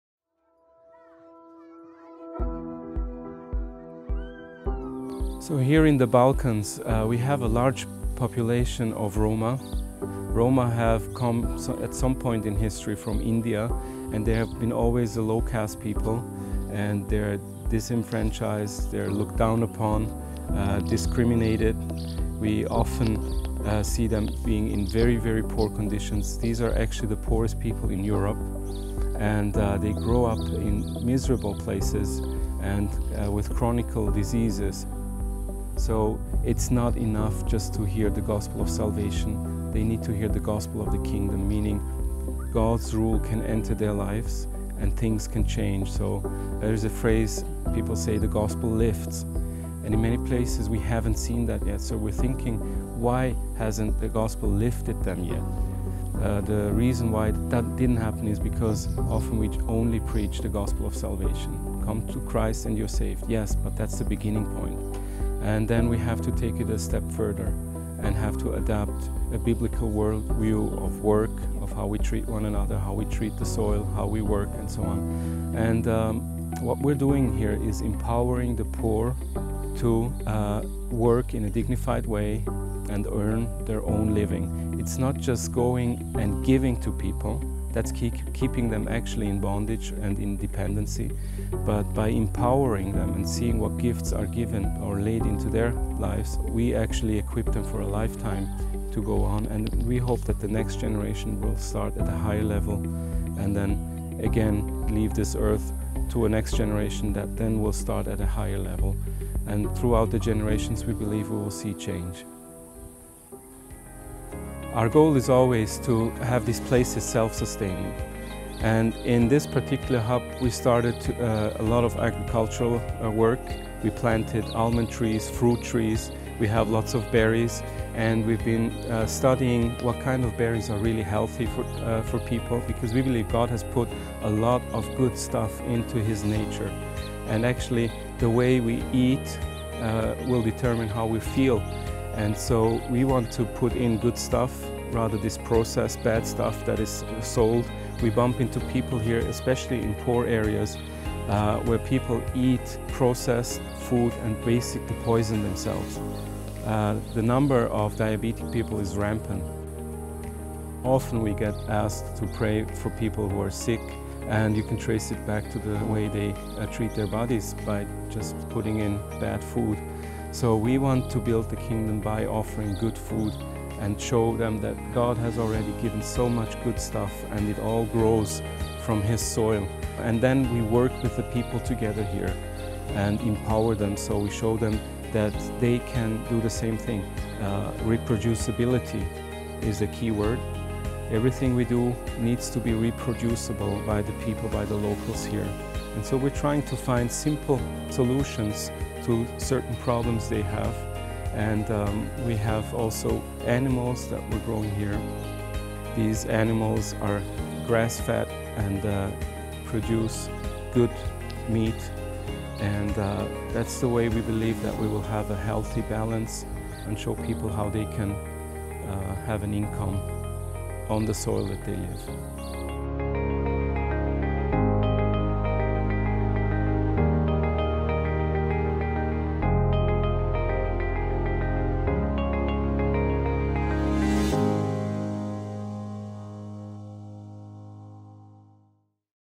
Zentralasien – Die Turkvöllker auf dem Herzen
Hier auf dem Balkan gibt es viele Romas. Die Romas kamen ursprünglich aus Indien. Sie galten schon immer als minderwertige Menschen ohne Rechte. Man schaut auf sie herab, sie werden diskriminiert und leben oft in sehr armen Verhältnissen. Es sind die ärmsten Menschen in Europa. Sie wachsen in elenden Orten auf, es gibt viele Krankheiten.